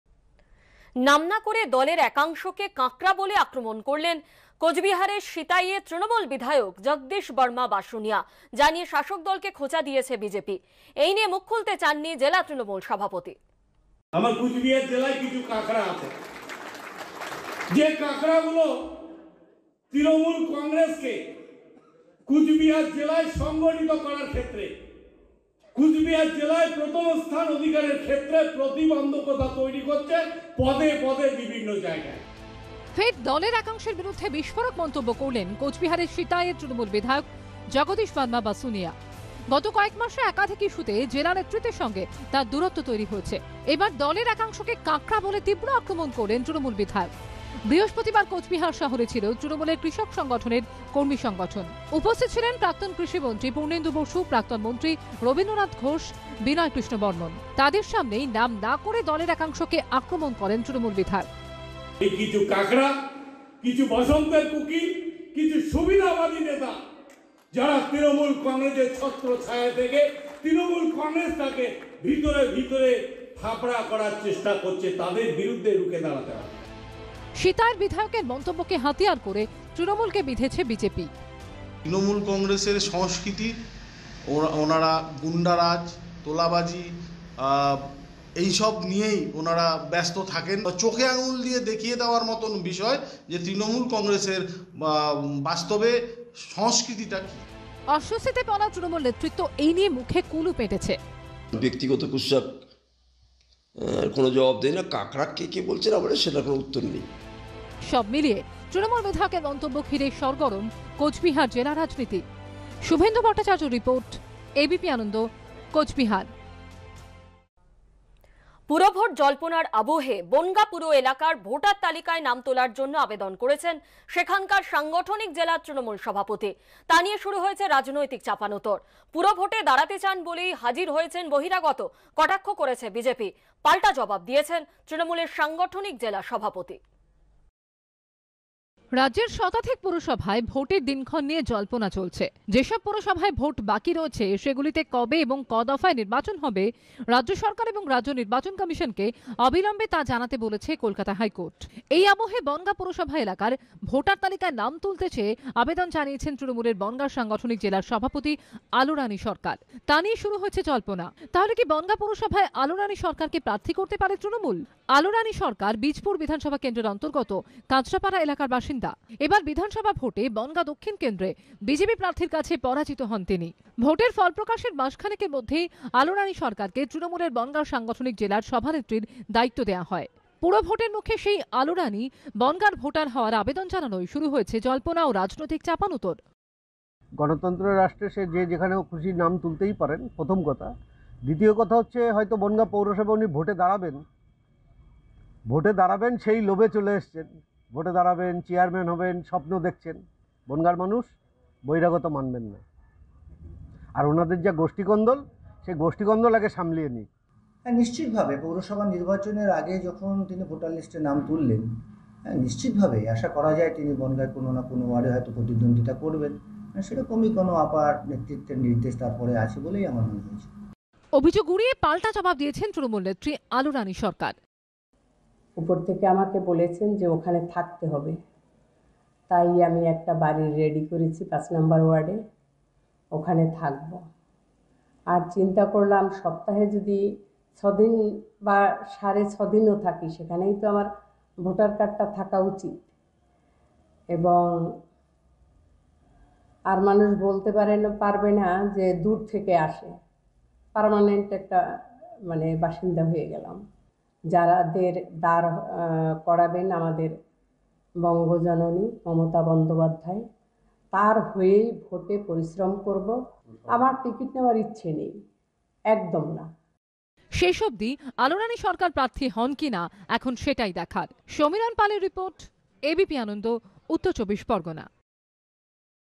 WB Politics: দলের একাংশকে 'কাঁকড়া' বলে নিশানা জগদীশ বর্মা বাসুনিয়ার | Bangla News
নাম না করে ,দলের একাংশকে কাঁকড়া বলে আক্রমণ করলেন কোচবিহারের সিতাইয়ের তৃণমূল বিধায়ক জগদীশ বর্মা বাসুনিয়া (Jagadish Chandra Barma Basunia)। যা নিয়ে শাসক দলকে খোঁচা দিয়েছে বিজেপি। এই নিয়ে মুখ খুলতে চাননি জেলা তৃণমূল সভাপতি।